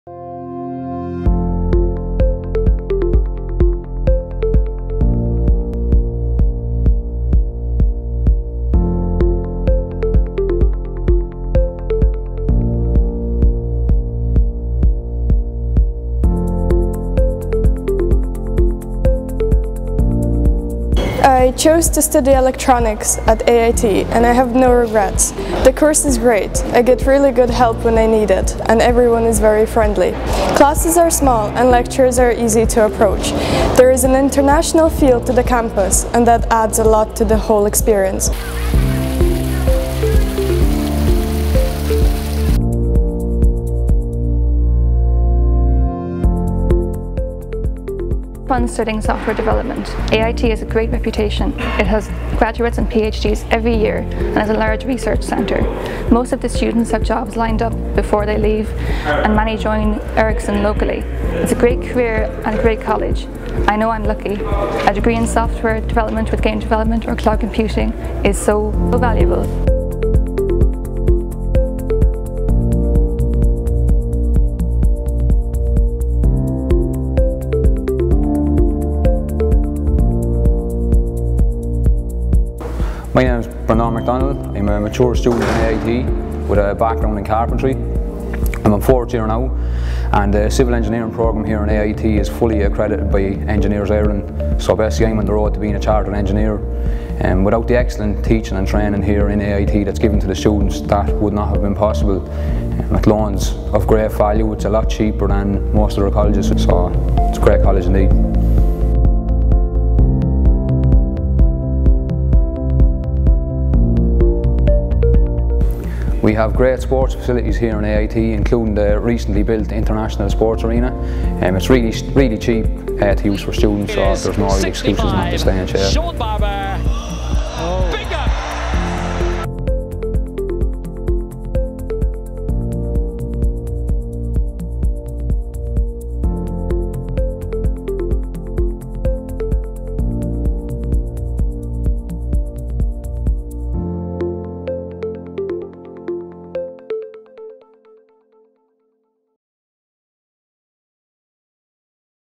Discover Engineering at AIT
CAO 2016: Discover Engineering at AIT. The Faculty of Engineering incorporates the disciplines of construction studies, civil, mineral, mechanical/manufacturing, mechatronics, plastics/polymer, electronic, computer and software engineering.